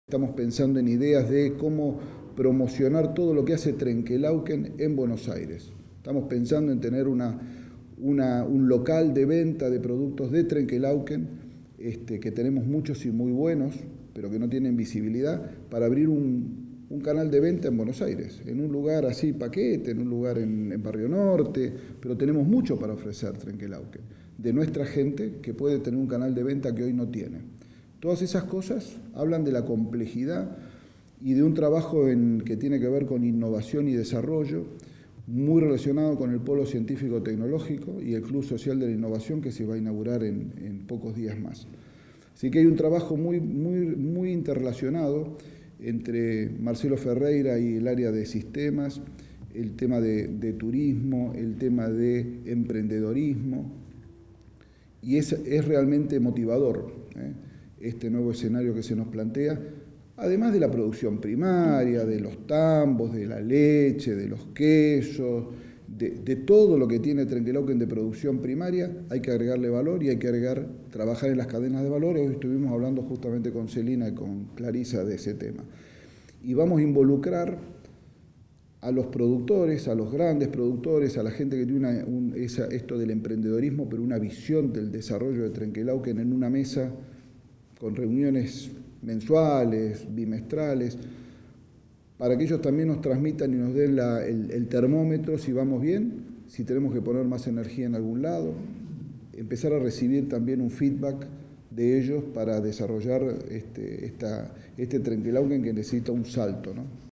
El municipio buscará promocionar productos locales en Buenos Aires
Según explicó el intendente Miguel Fernández, una meta es “acompañar a la industria primaria y a los comercios”. Será una de las tareas de Clarisa Fabris, recientemente designada al frente de la Secretaría de Producción.